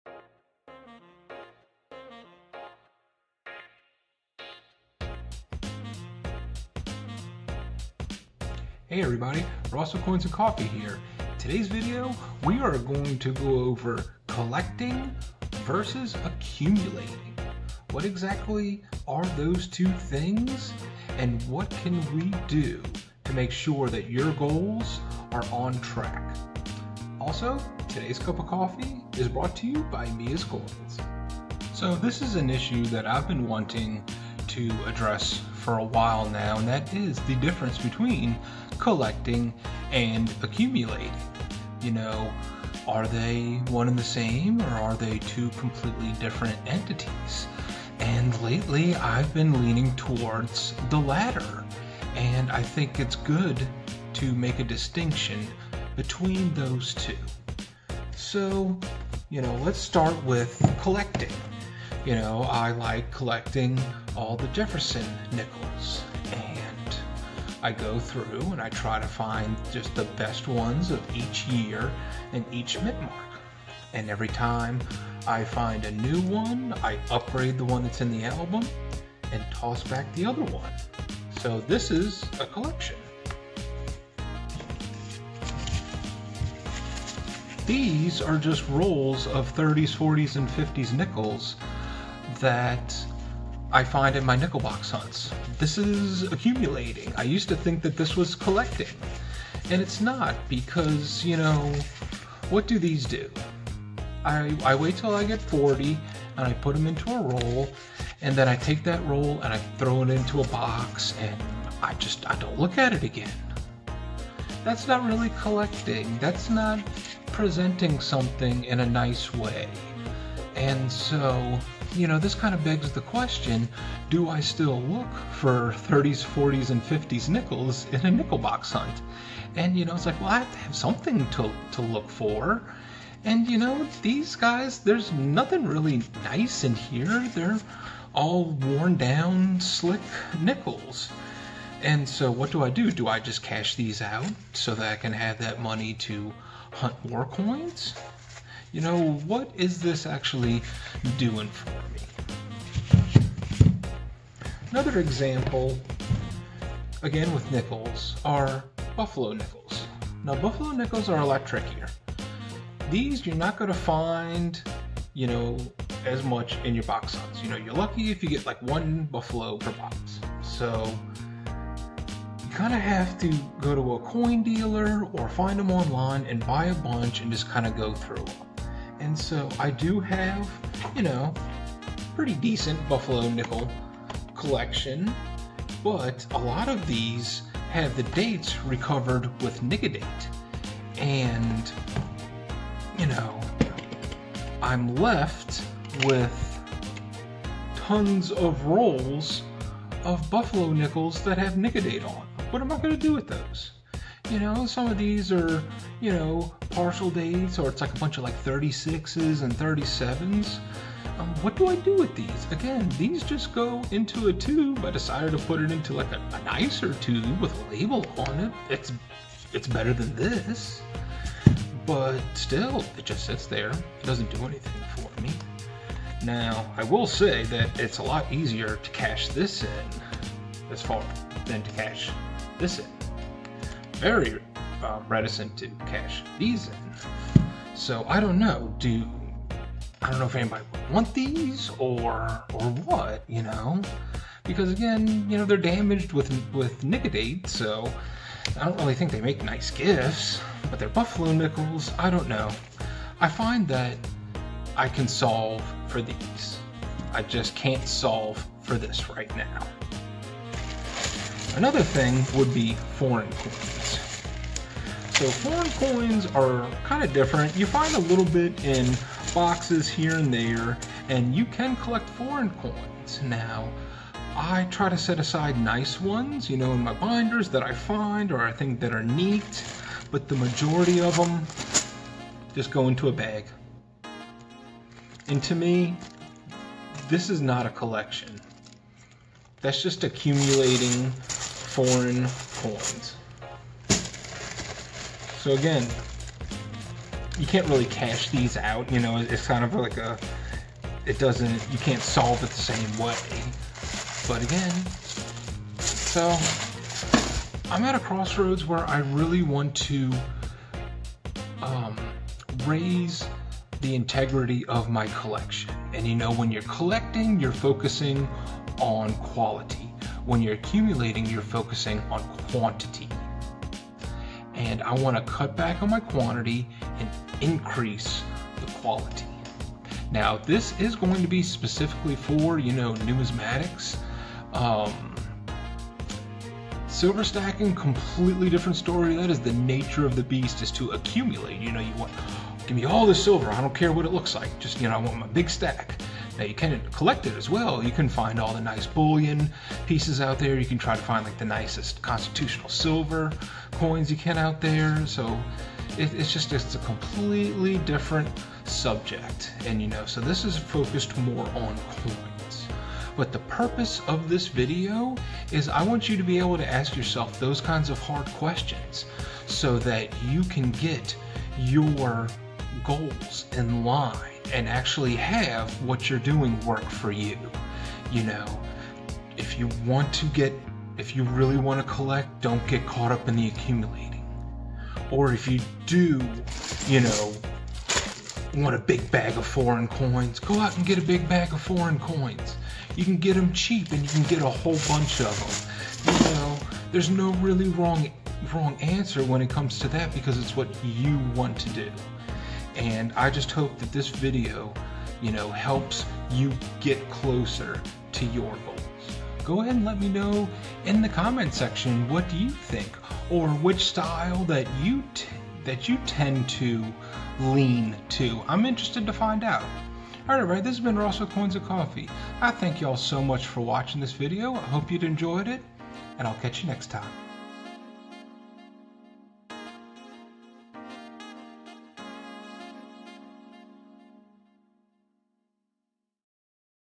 Collections Vs. Accumulation | Are Your Goals In Line?
Episode #119

Hey everybody,

Today we're going to ask ourselves the hard questions about what our goals are and if we honestly adhere to those goals. Remember to always do what works for you, but I hope this video helps shed some light for some as it did for me. Enjoy!